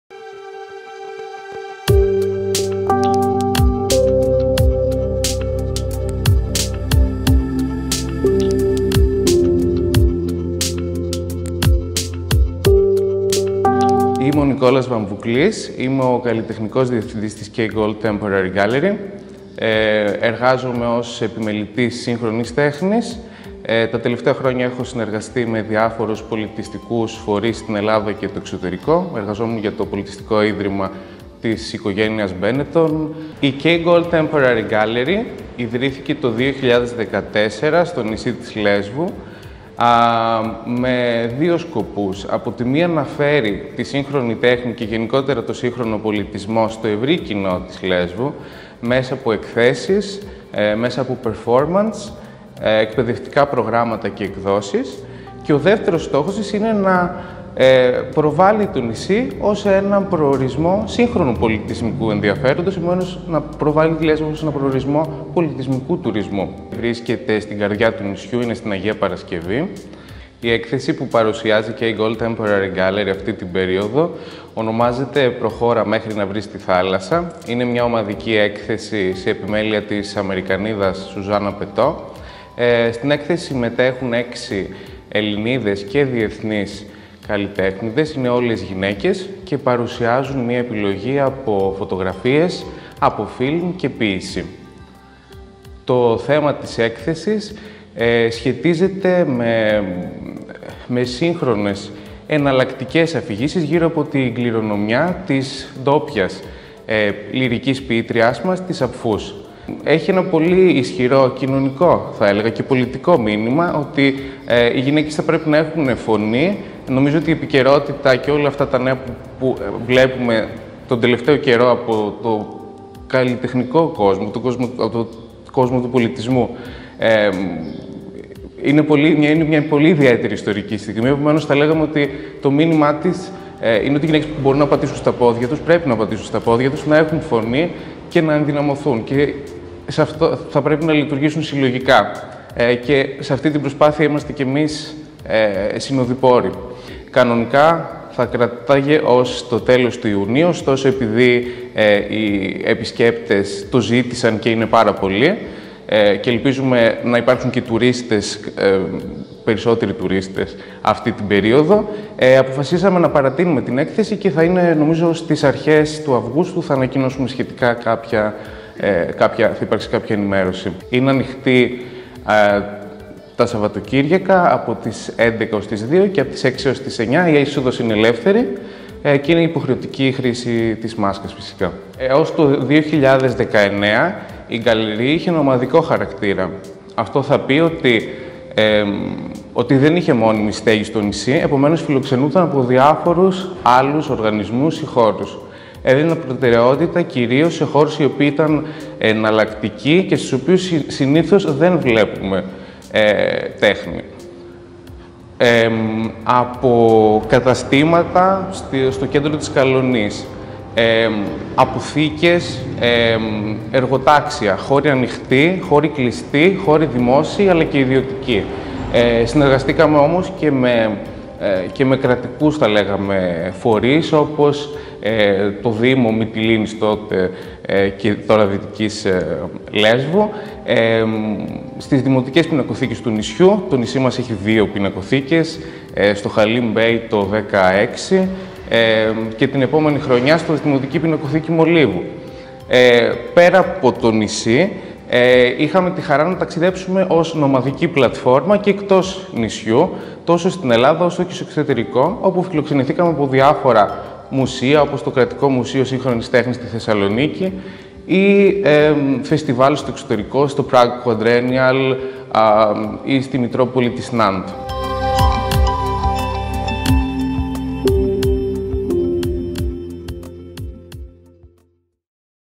Ένας ναός σύγχρονης τέχνης στην Αγία Παρασκευή
Συζητώντας για την K-Gold Temporary Gallery  με τον Νικόλα Βαμβουκλή